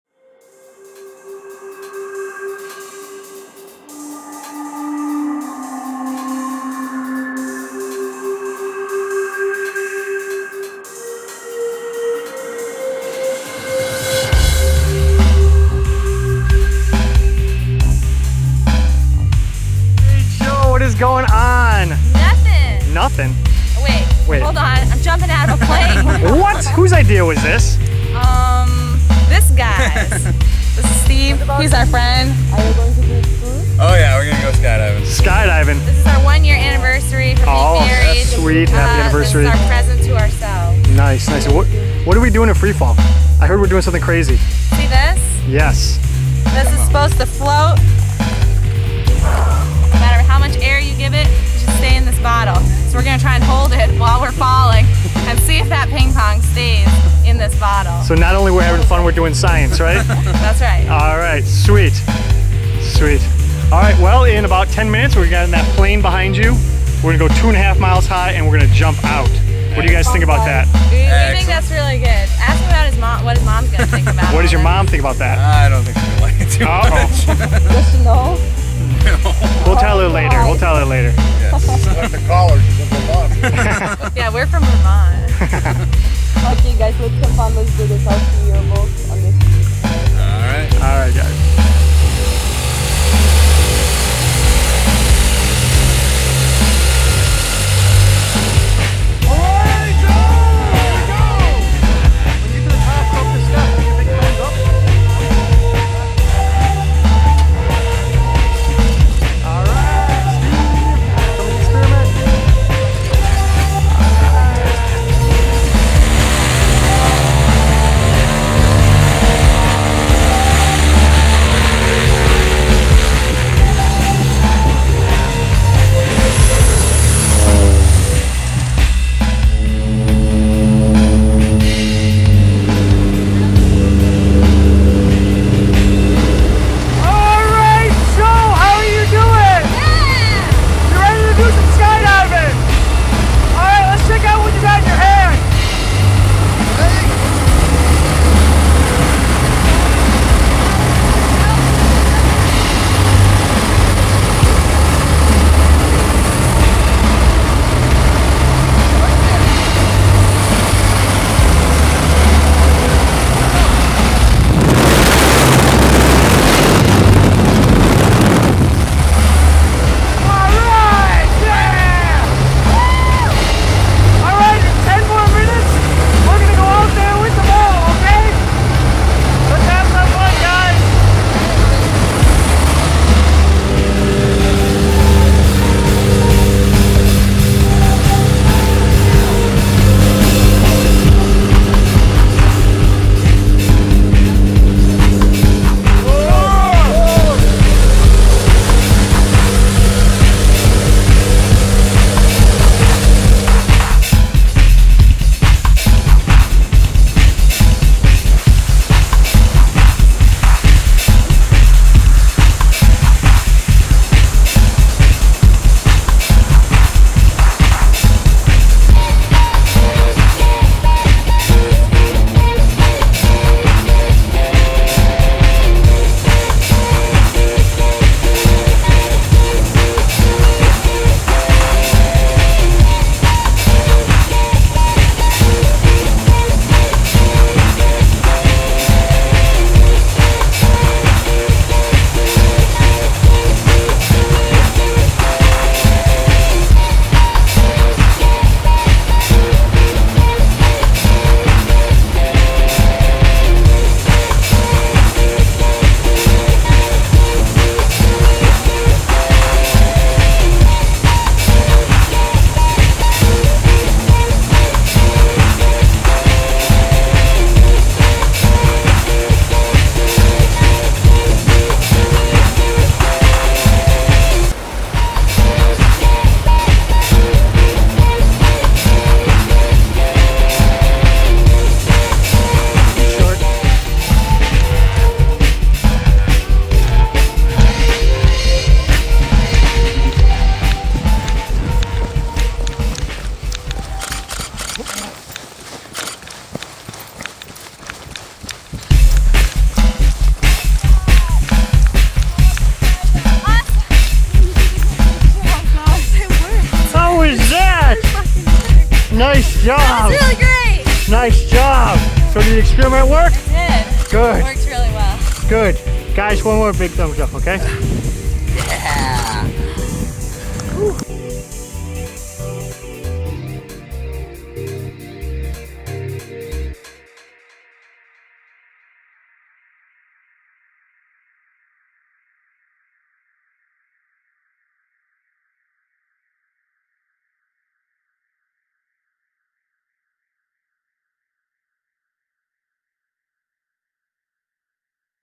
Tandem Skydive Experiment
This is my first tandem skydive. While rushing towards the ground at approximately 120 mph, I will demonstrate that 'Moving Air Follows a Curve' using a ping pong ball and a plastic bottle with no cap and no bottom. This experiment worked best when we held it out of the plane but it also worked in short spurts during our fall. You can do this at home with a cone held vertically and a ping pong ball. Blow as hard as you can; the ball won't come out. Or you can set it up like we did before our jump with a soda bottle (modified to have no cap on and no bottom), a ping pong ball and an air compressor. It still won't come out as long as the air is steady and is passing all the way around the ball. Thank you to the staff and jumpers at Skydive Tecumseh for film editing and making this film possible.